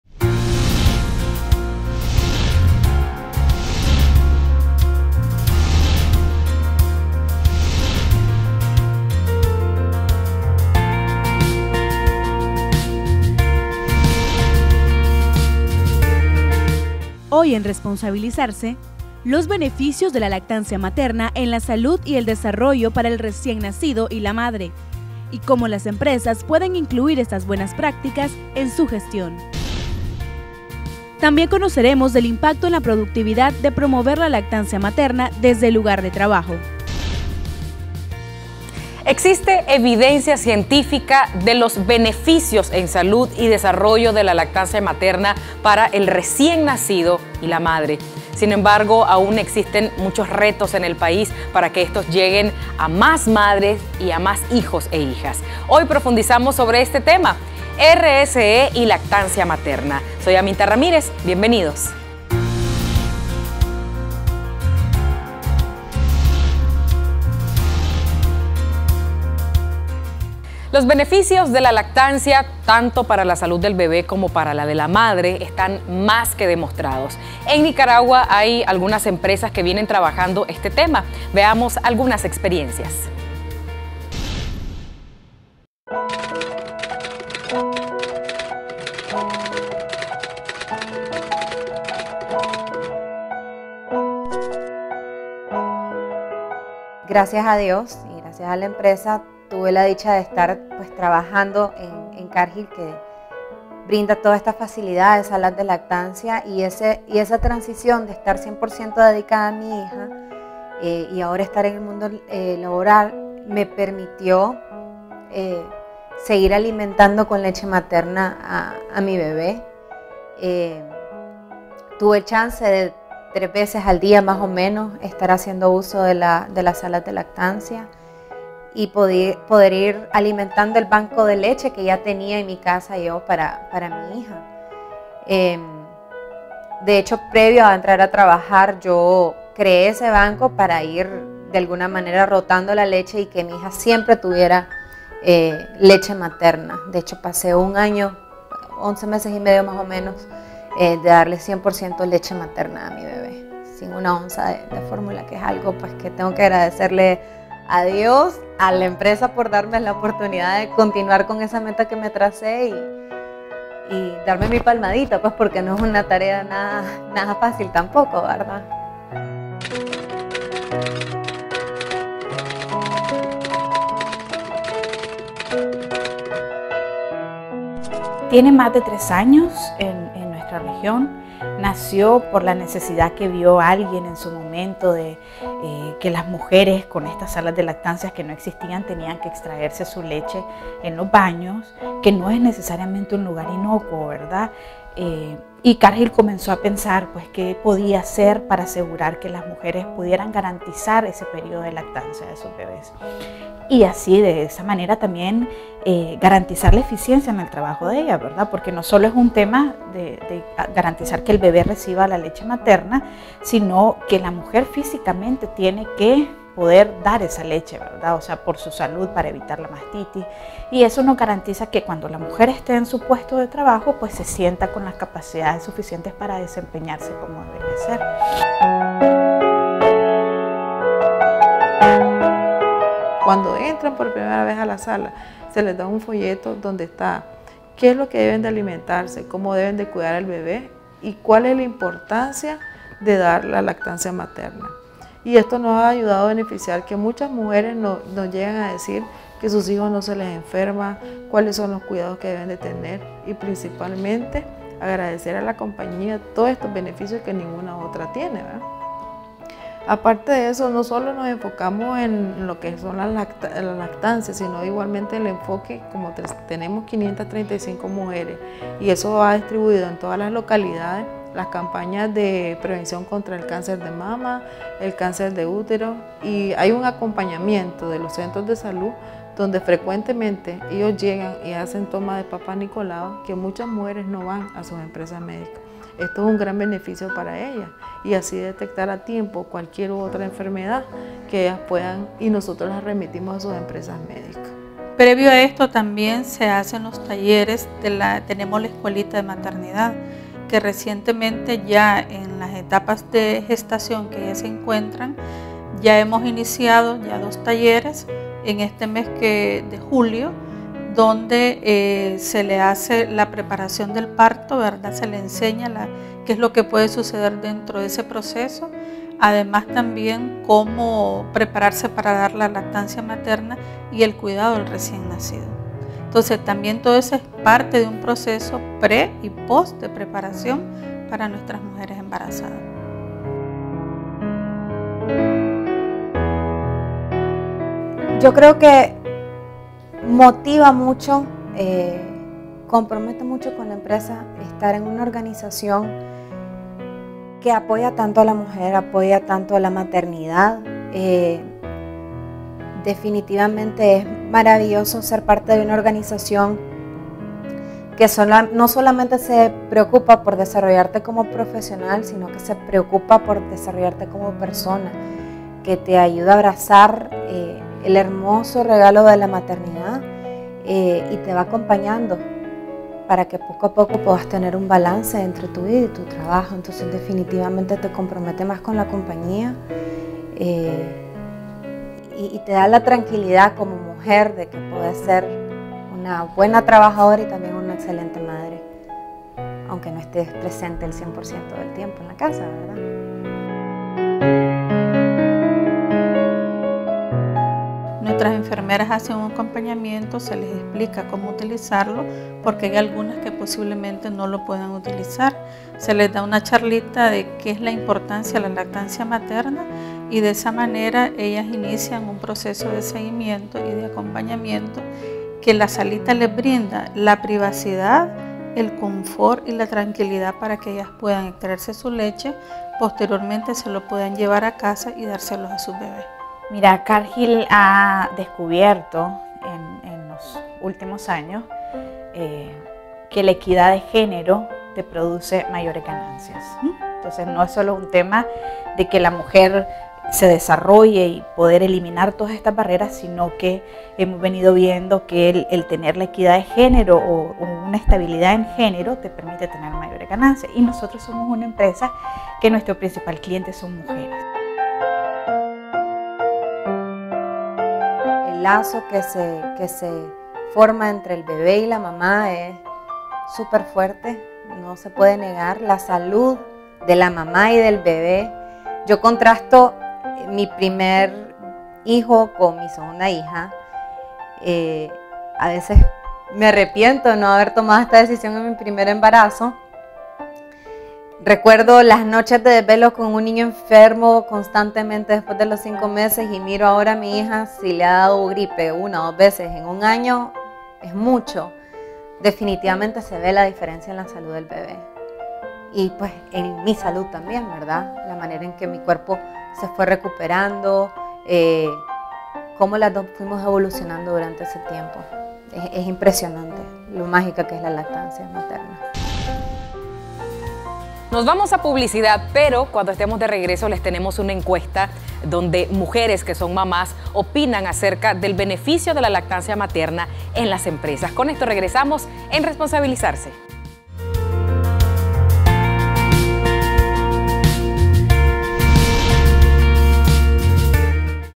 Cargill desarrolla prácticas a favor de la lactancia materna
La creación de las salas de lactancia materna es parte de las acciones que desarrolla Cargill, para que las mujeres que desempeñan tareas en la empresa logren su máximo potencial profesional, personal y a la vez contribuir a las barreras que afrontan para alcanzar su éxito.